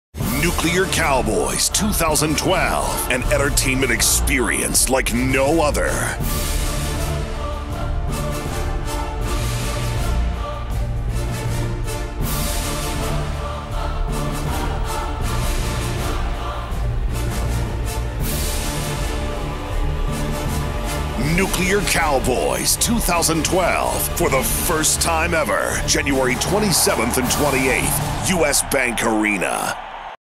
Nuclear Cowboyz - First Time Ever at U.S. Bank Arena - January 27th & 28th
2012 Nuclear Cowboyz -- An entertainment experience like no other!  Visits U.S. Bank Arena for the first time ever on January 27th and 28th, 2012!

Legendary Nuclear Cowboyz FMX tribes, the Soldiers of Havoc and the Metal Mulisha® return for a never-before-seen action-packed event that takes place in the war torn city of Los Angeles during the year 2150.  The tale unfolds as the two tribes' survival is threatened and besieged by an evil force, The Tempest and her electrifying Cyborg Army. Catapulted into the heart pounding fury and frenzied excitement are the sizzling Nuclear Cowgirlz®, whose allegiance to their tribes only intensifies the Tempest's rage to destroy the Nuclear Cowboyz. The Soldiers of Havoc and the Metal Mulisha's battle is fought through fearless freestyle gravity defying stunts, combined with outrageous pyrotechnic and laser displays synchronized to heavy metal, rock alternative and electronic dubstep music.

Nuclear Cowboyz 2012 has the greatest cast of freestyle motocross, trials and quad riders ever assembled, including recent X Games 17 medalists Ronnie Faisst, Adam Jones, Mike Mason, and Winter X Games medalist Caleb Moore. Joined by world-class medalist riders Taka Higashino, Beau Bamburg, Derek Garland, Nixey Danielson, Brody Wilson, Jimmy Fitzpatrick, Dustin Miller, Wes Agee, Matt Buyten, Geoff Aaron, Keith Wineland, Derek Guetter, and Colton Moore, NuclearCowboyz 2012 takes unleashed power to its massive ramps in a freestyle performance of pure danger and raw combat.